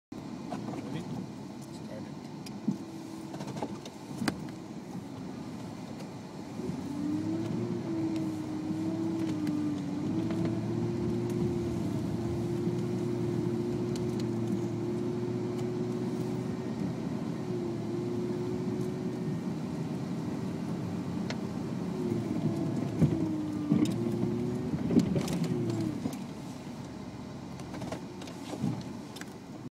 BOSS Plow 2012
Plowing snow with 98 GM Suburban K2500 w/ 9.2' BOSS snow plow January 11, 2012